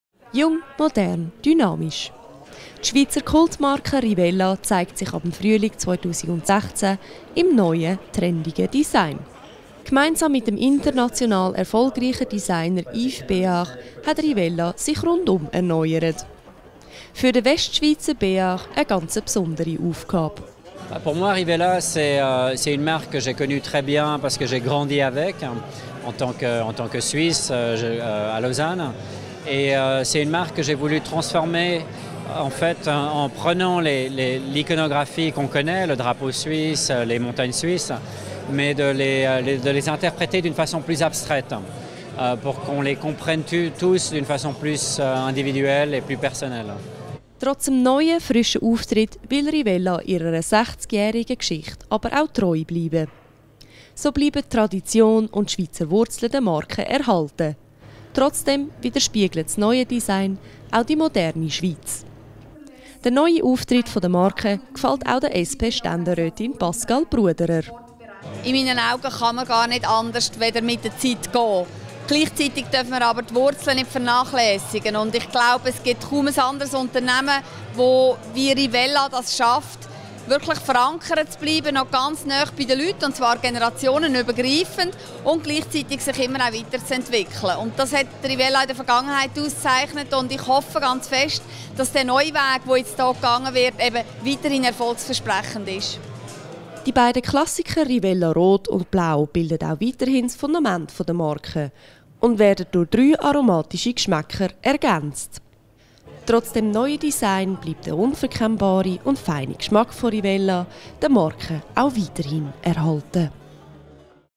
Eine neue Rivella-Ära beginnt
Zürich - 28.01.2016 - Ab Frühling 2016 präsentiert sich Rivella mit neuem Design. Gemeinsam mit dem international erfolgreichen Designer Yves Béhar hat die Rivella AG den Auftritt der Schweizer Kultmarke rundum erneuert. (PR-Beitrag)